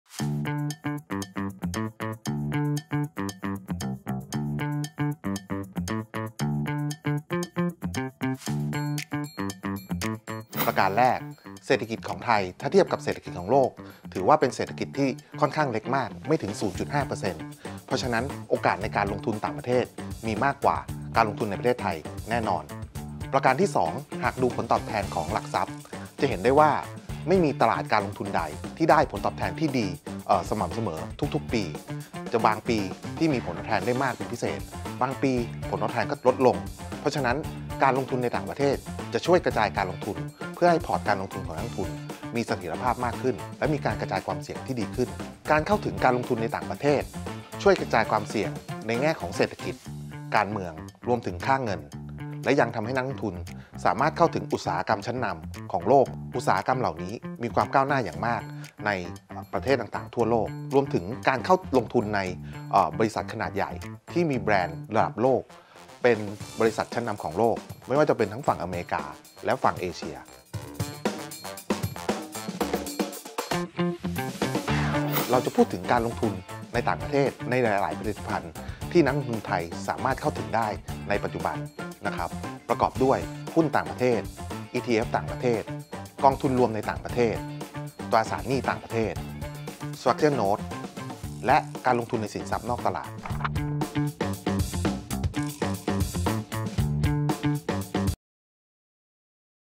Highlight : อยากลงทุนต่างประเทศ ลงทุนอะไรได้บ้าง
ทำไมต้องกระจายการลงทุนไปยังต่างประเทศ?
ผลิตภัณฑ์ที่ช่วยกระจายการลงทุนไปยังต่างประเทศ มีอะไรบ้าง?

มาสำรวจทางเลือกใหม่ๆ ที่หลากหลาย ไม่ว่าจะเป็นหุ้น กองทุน ETF หรือสินทรัพย์อื่นๆ จากทั่วโลก พร้อมเรียนรู้ข้อดี-ข้อเสียของแต่ละทางเลือก เพื่อสร้างพอร์ตลงทุนที่แข็งแกร่งและหลากหลายมากยิ่งขึ้น พร้อมรับมือกับทุกความท้าทายในตลาดโลก ได้ที่